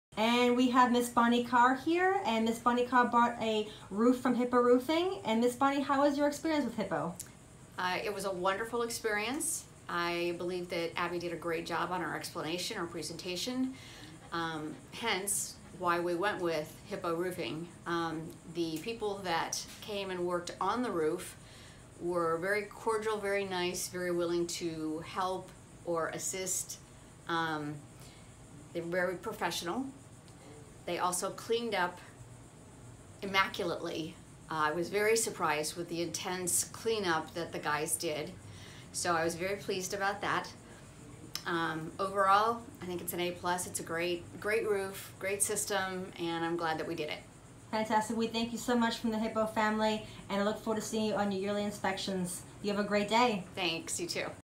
Hippo Roofing Testimonials & Reviews- Melbourne, FL
Hippo Roofing, your premier Melbourne, Florida roofing company & home of "Metal Roofs Same Price As Shingles."

In this video you will hear from a past client describing their recent re-roof done by Hippo Roofing.

"It was a great experience with Hippo Roofing. The roofers were very nice, very cordial & cleaned up immaculately”!